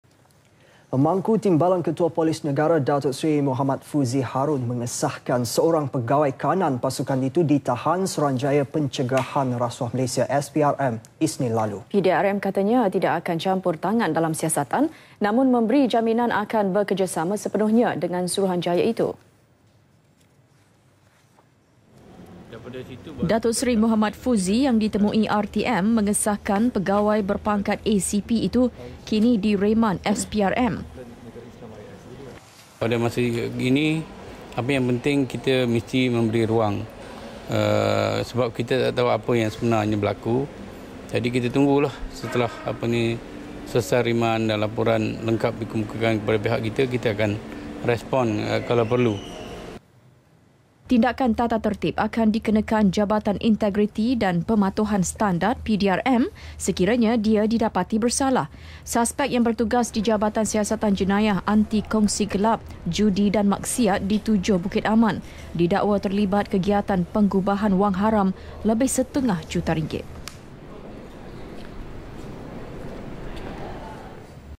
PEGAWAI KANAN PDRM DITAHAN SPRM [12 Nov 2014]
Pemangku Timbalan Ketua Polis Negara, Datuk Seri Mohamad Fuzi Harun mengesahkan seorang pegawai kanan pasukan itu ditahan Suruhanjaya Pencegahan Rasuah Malaysia (SPRM) Isnin lalu.

PDRM katanya tidak akan campur tangan dalam siasatan, namun memberi jaminan akan bekerjasama sepenuhnya dengan suruhanjaya itu.